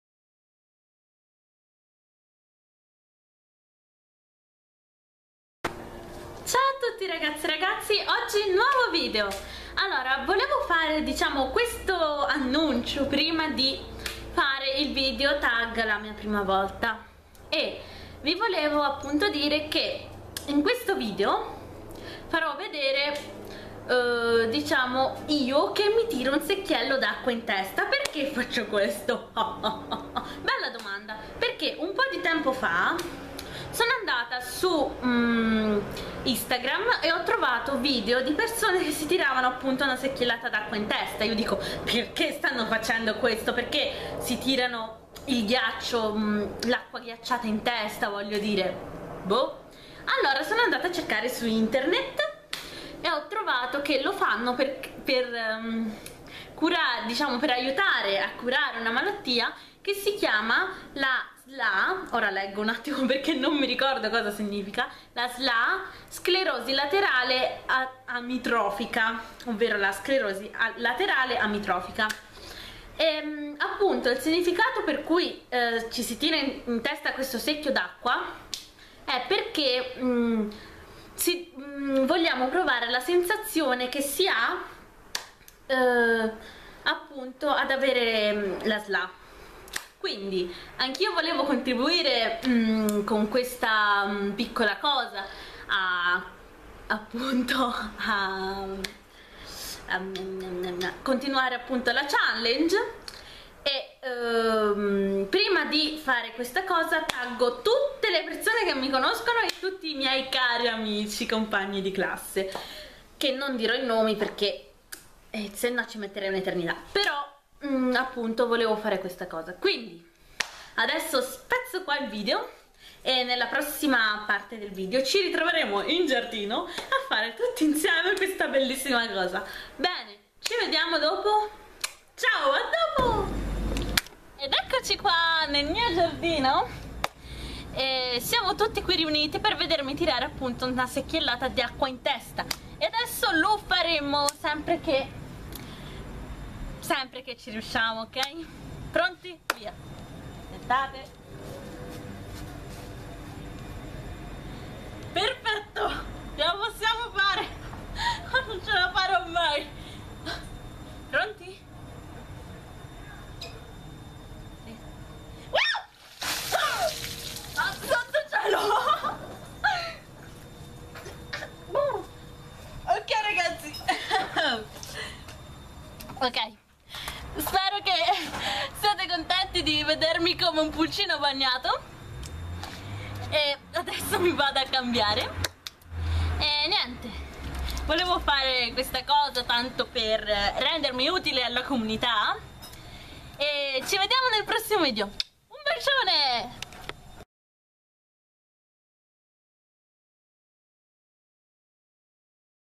video per la SLA
le persone colpite da SLA molte volte fanno fatica ad essere curate...AIUTIAMOLE! doniamo un euro per questa malattia. ringrazio Elena ed Ester per avermi dato l'idea...vi ringrazio per aver guardato questo video! iscrivetevi al canale!..eeee. un bacione a tutti ! al prossimo video!